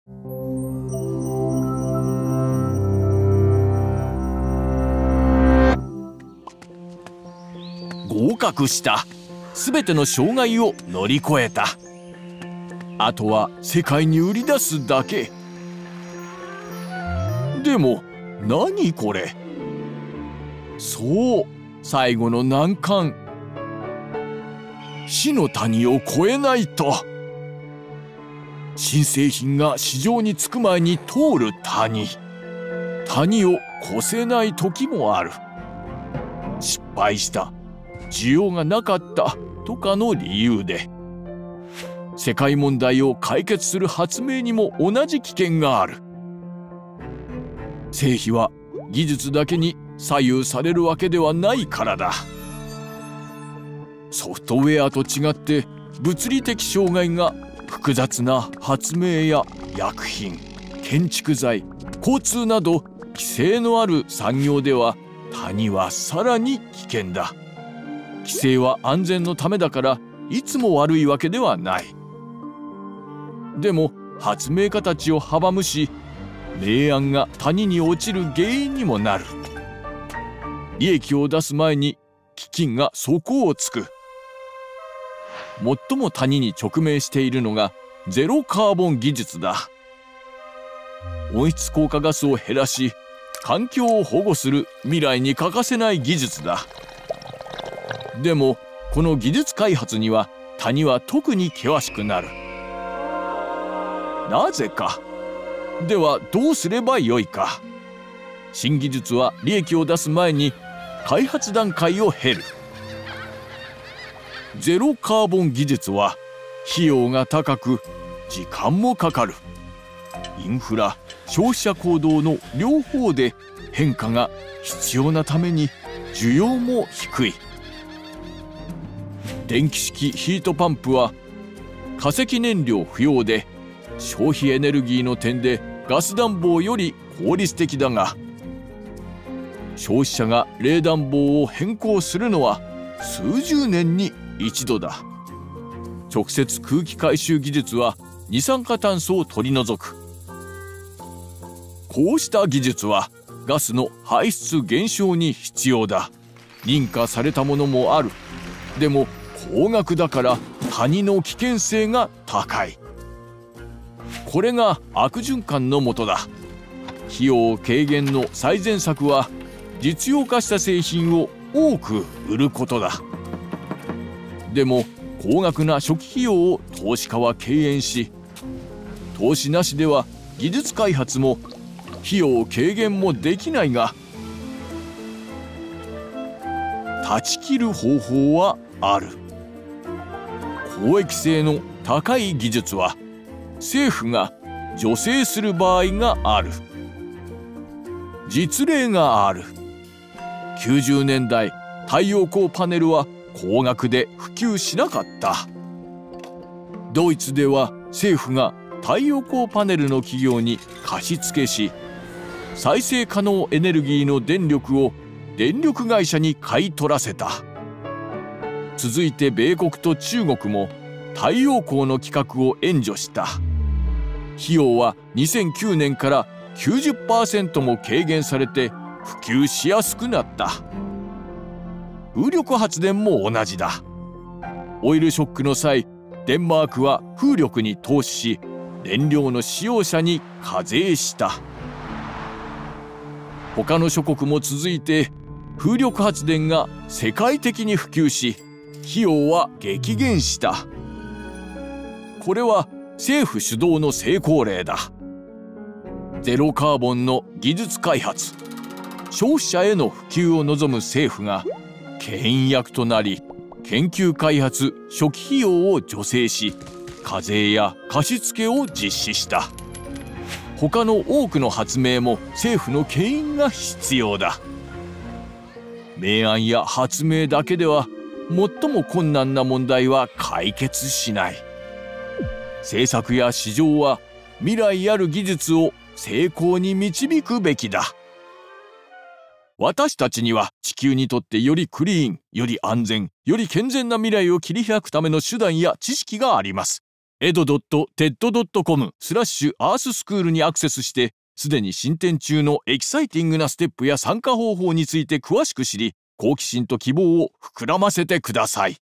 なぜ優れたアイデアが「死の谷」に落ちるのか？どうすれば救済できるのか？
新製品は、市場に辿り着く前に「死の谷」を通り過ぎなければなりません。多くは谷を越せすことができず、失敗した 需要がなかったなどの理由であれば、それでも構わない場合もあります。
この問題が最も差し迫っている分野の一つは、ゼロカーボン技術です。なぜ、この落とし穴に落ちやすいのでしょう？それを変えることができるのでしょうか？資金不足のサイクルを断ち切る方法を見てゆきましょう。

Lisa LaBracio 監督。